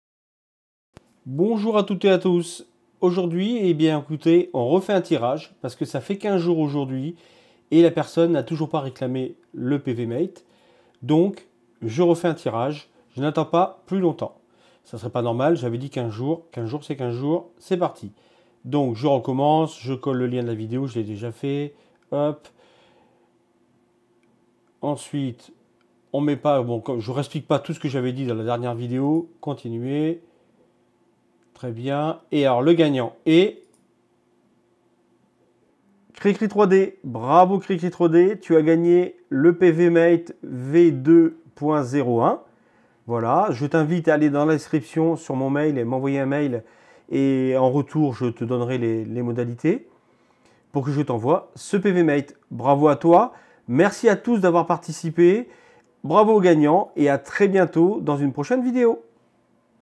PVmate deuxième tirage
Voici la vidéo du tirage au sort du "nouveau" gagnant du PVmate.